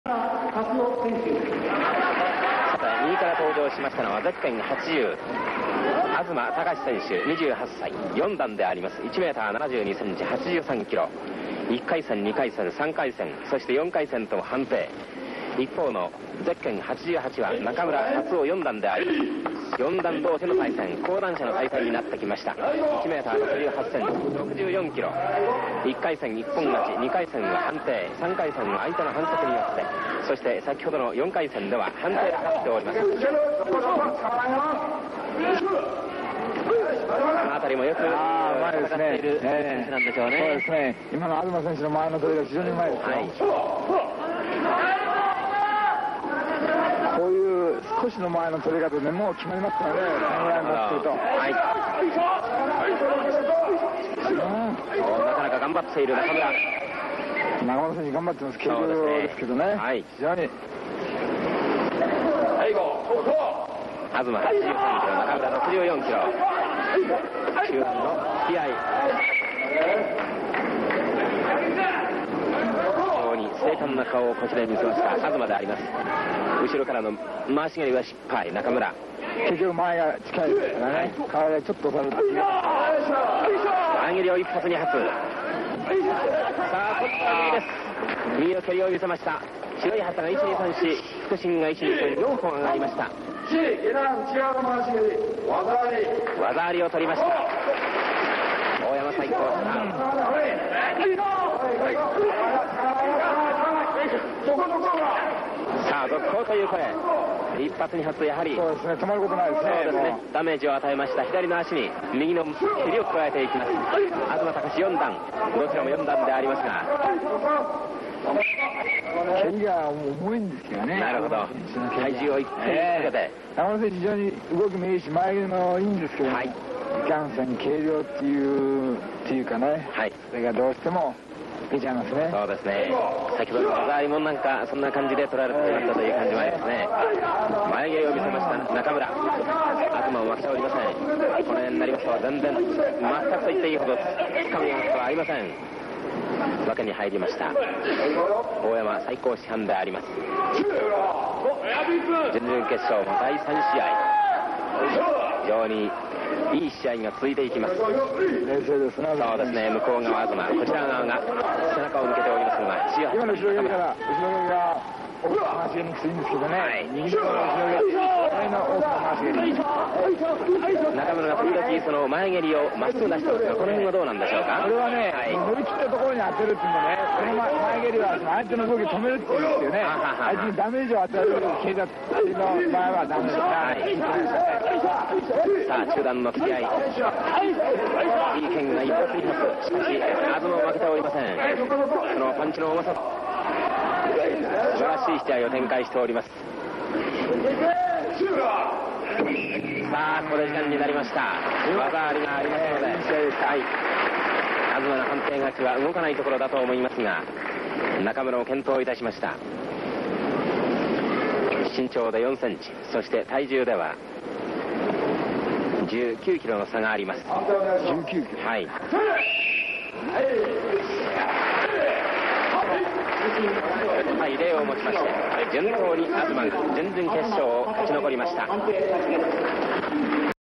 Takashi Azuma vs Tatsuo Nakamura (1977)
Quarterfinal the 9th All Japan Open Karate Tournament (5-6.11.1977)
Takashi Azuma (80) - Win (wazari - lowkick)